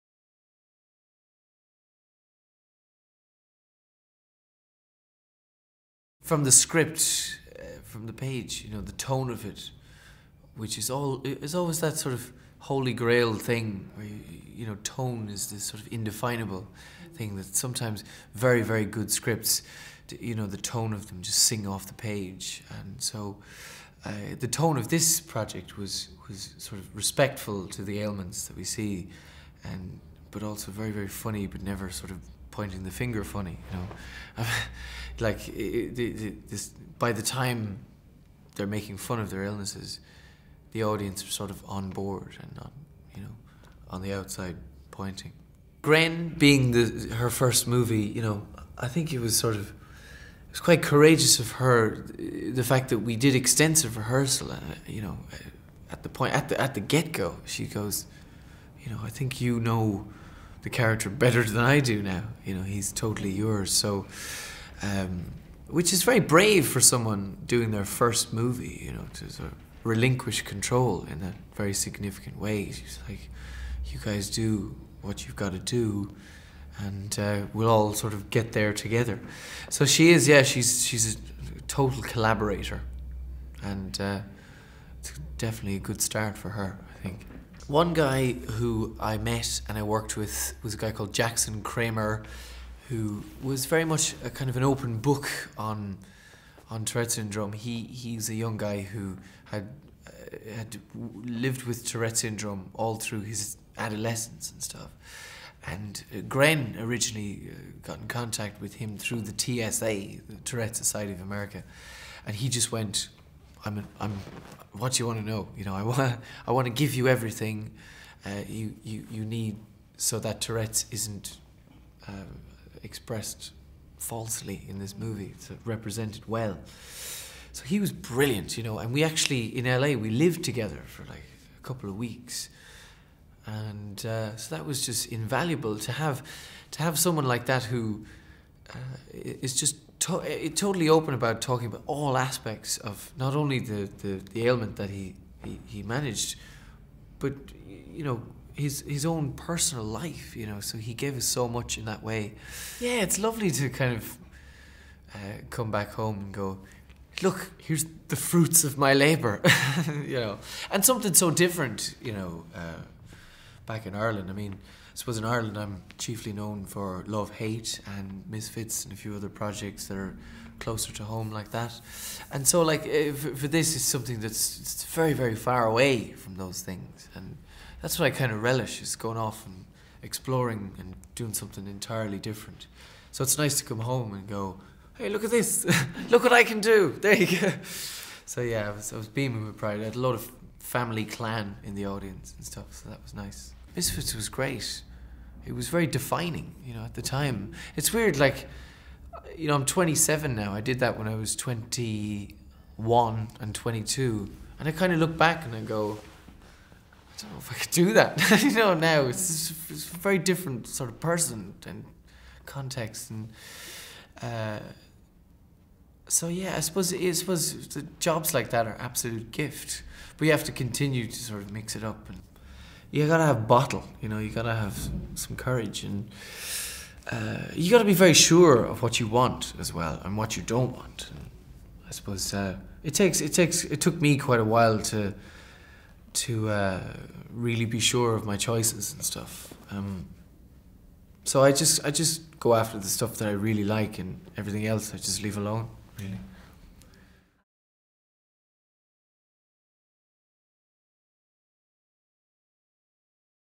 Festival 2015 | In Conversation | Robert Sheehan
In Conversation with Robert Sheehan who was our special guest at this years festival!